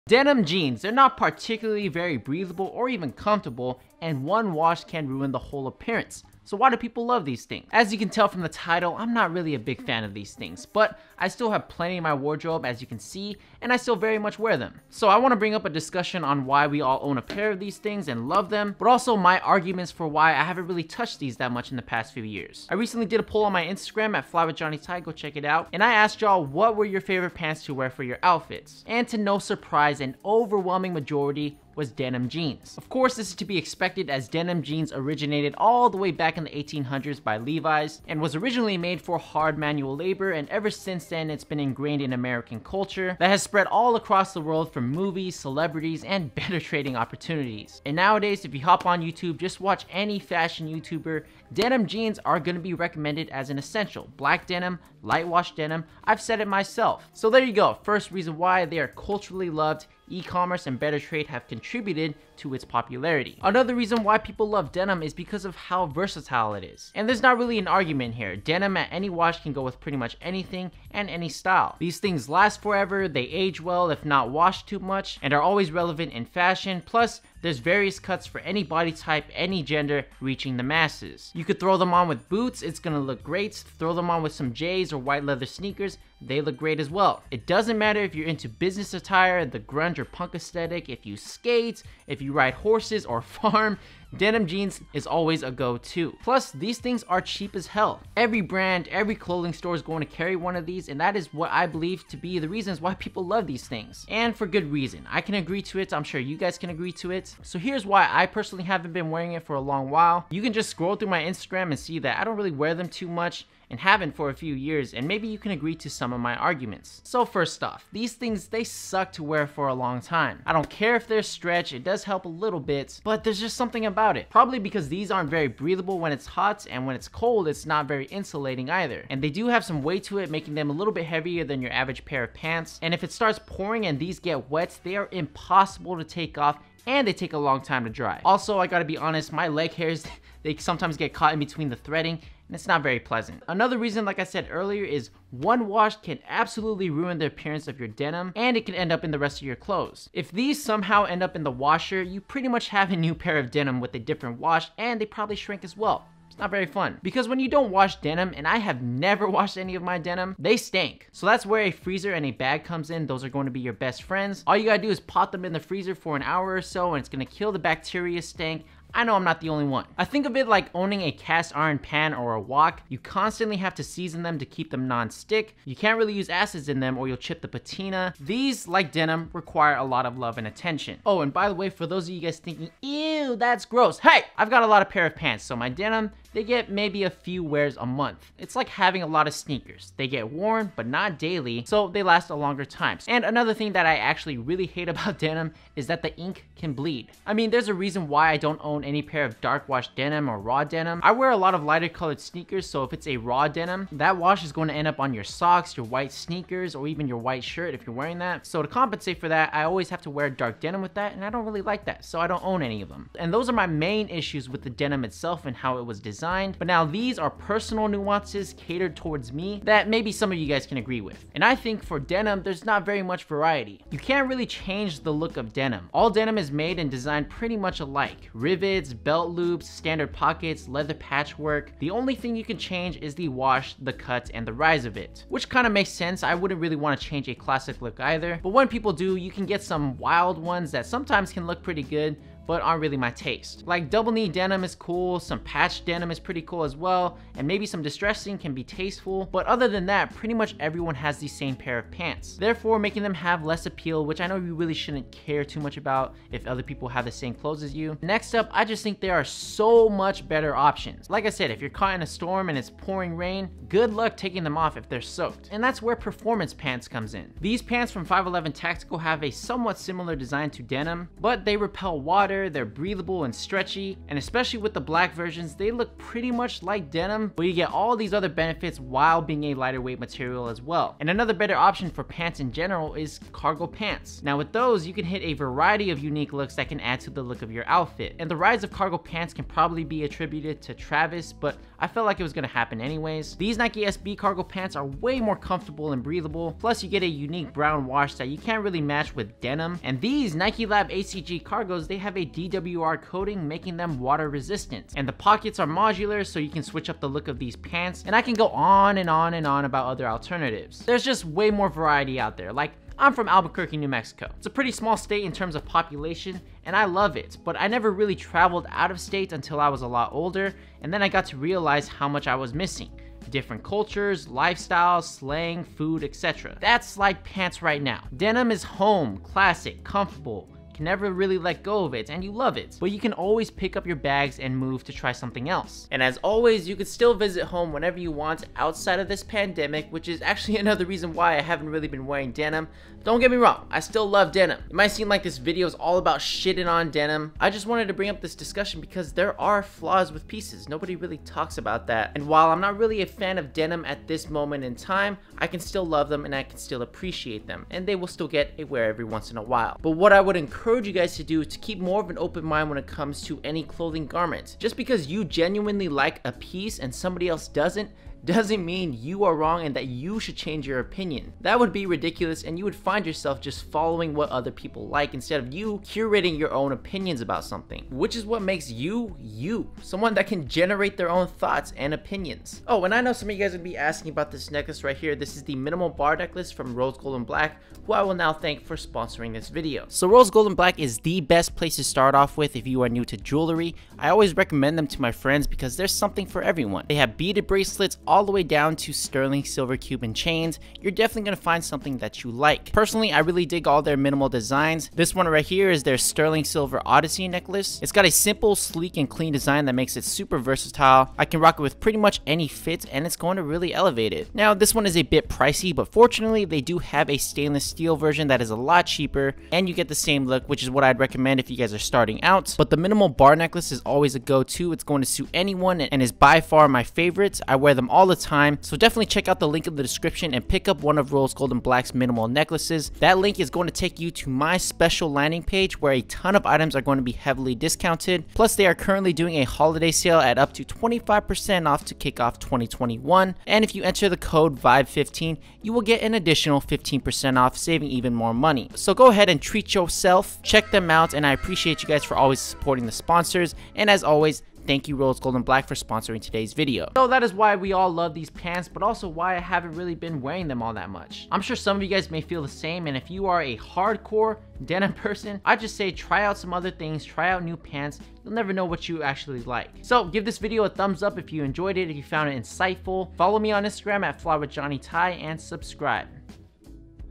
Why People LOVE Denim Jeans (and Why I'm On The Fence...)
This is why I don't like denim but also do. The title and format inspiration came from Adam Ragusea’s opinion on cast iron pans

5.11 Tactical Apex Cargos
Nike SB Flex Cargo Pants

All of my camera gear I use for Instagram and videos:

My office setup for background and work: